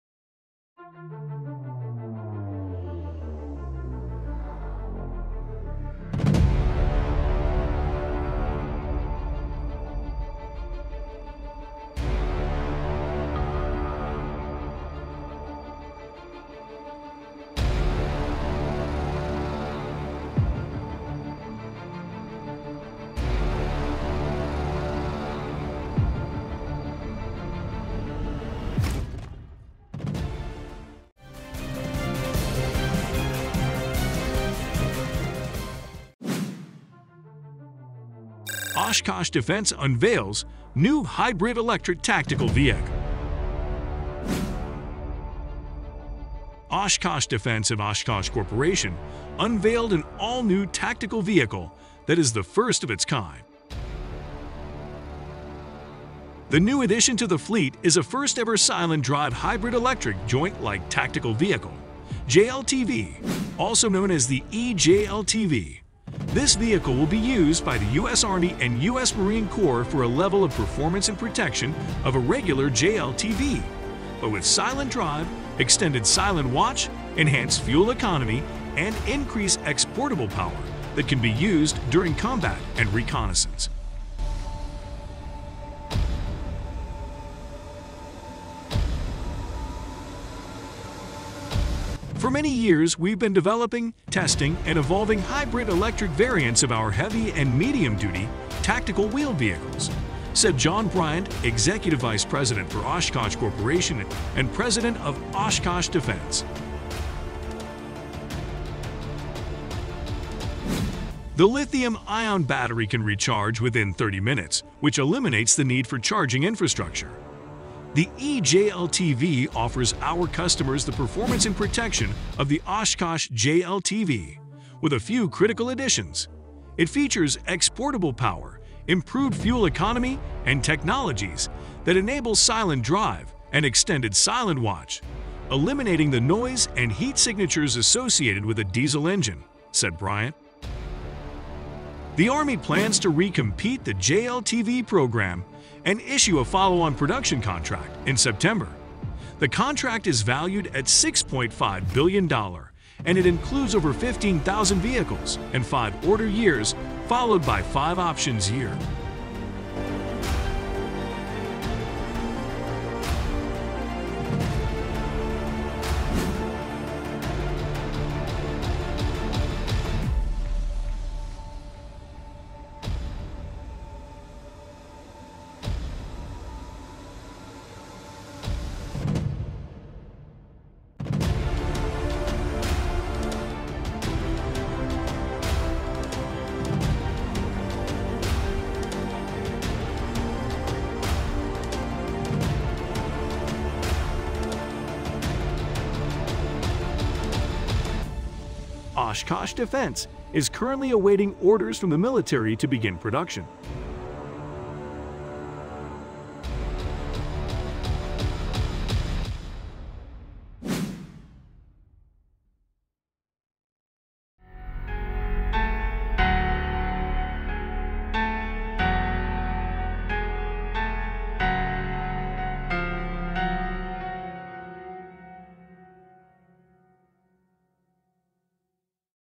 New Version: Oshkosh Defense unveils new hybrid-electric tactical vehicle
latest news
Oshkosh Defense has developed a new version of its Joint Light Tactical Vehicle featuring hybrid-electric technology, which executives say will offer users increased capabilities and improved fuel economy.

Oshkosh's new platform ­— known as the eJLTV — offers military customers exportable power, improved fuel economy, and silent drive and extended silent-watch capabilities, said John Bryant, executive vice president of Oshkosh Corp. and president of Oshkosh Defense.